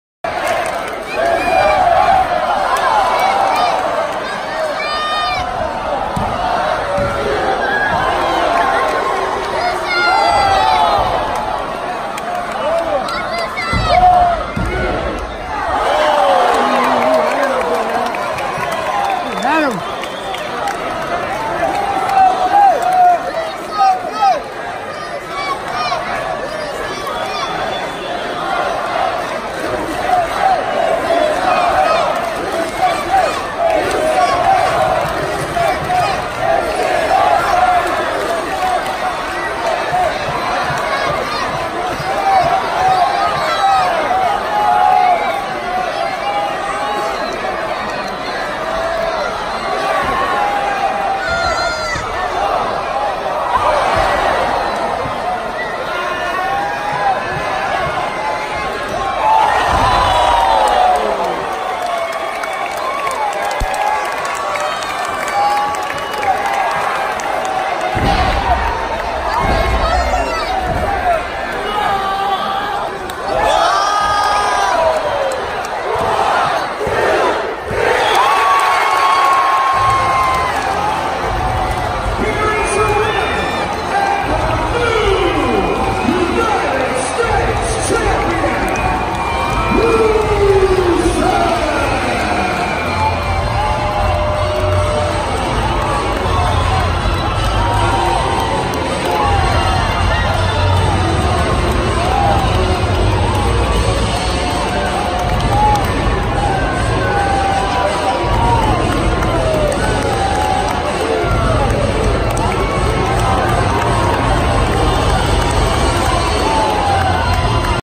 SPOILER!!! RUSEV WINS THE US TITLE. CHRISTMAS TAPINGS.
Rusev wins the US title at the Christmas tapings. Credit- @thenextbigthing Twitter.